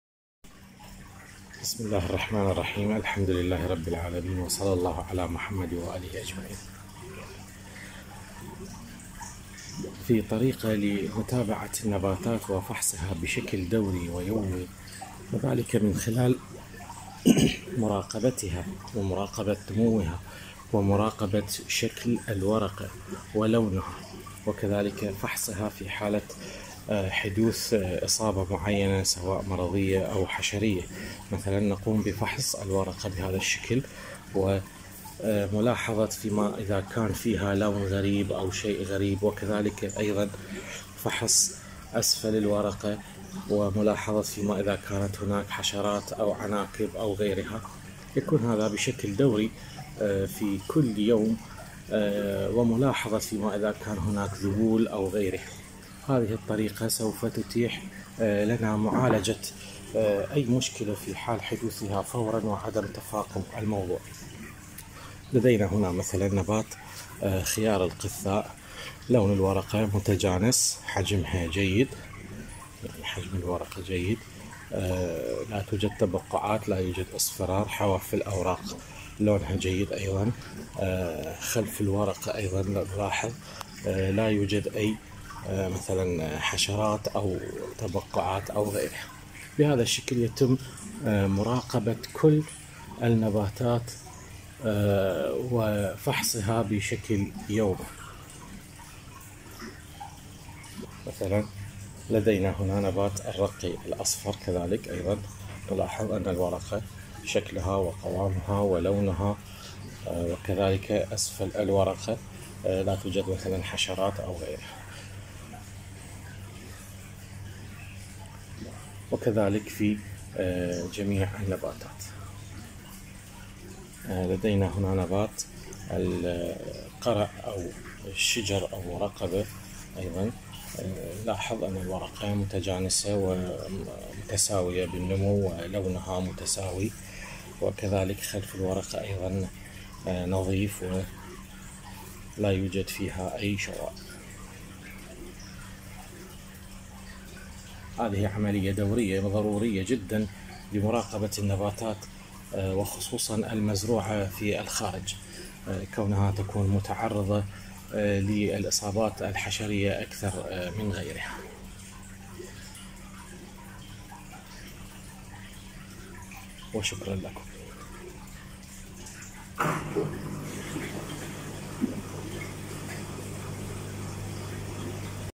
الفحص اليومي للنباتات في نظام الزراعة المائية
تعليم اساسيات الزراعة المائية